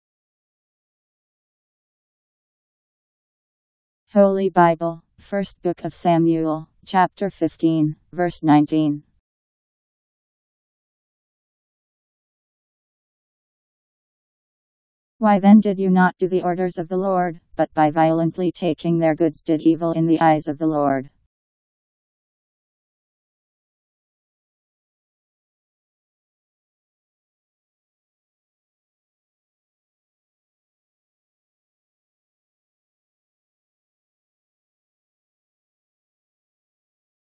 Holy Bible First Book Of Samuel 15:19
Video shows Holy Bible First Book Of Samuel Chapter 15 Verse 19: Why then did you not do the orders of the Lord, but by violently taking their goods did evil in the eyes of the Lord? Powered by MaryTTS. Text taken from Bible in Basic Edition. Bible First Book Of Samuel 15:19